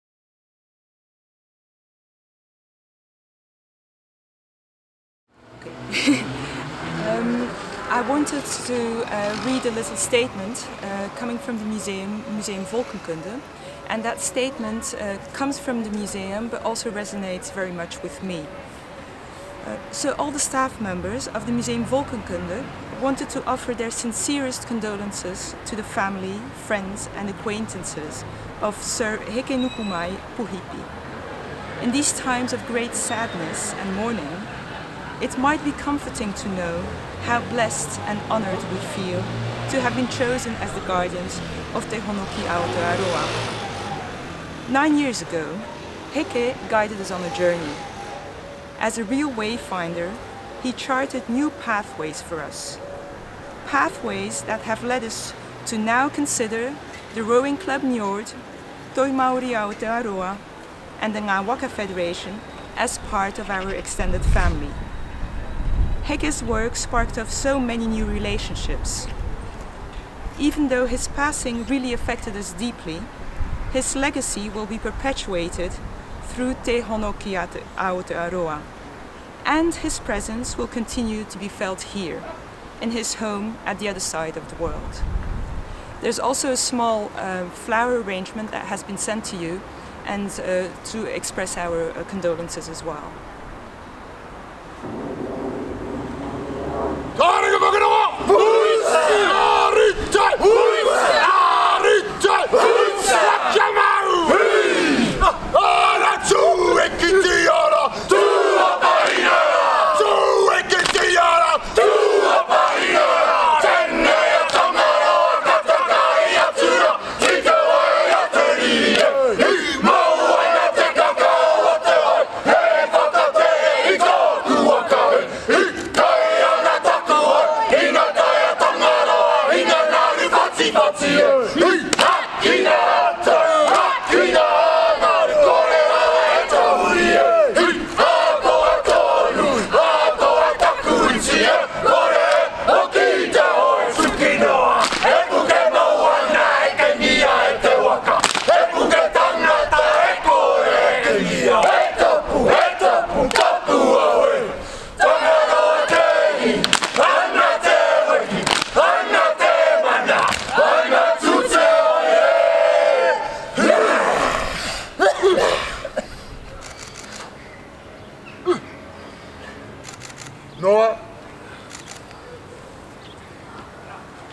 In commemoration of Sir Hekenukumai Busby
It is with great sadness in our hearts that we share the news that Sir Hekenukumai Busby (Uncle Hec) has passed away at the beautiful age of 86. He originally started his life as a bridge builder, but found that his real strength and passion lay with the carving of Waka and Polynesian navigation. He single-handedly revived Polynesian sailing and navigating and was a key figure in the Waka project. Without his trust and blessings we would not be paddling in Leiden today.

He meant a lot to the Maori Culture in New Zealand and the Waka will be a tribute to his legacy. By paddling in the waka he will continue to live on.

Our love and support goes out to his Whanau and everyone back in Aotearoa.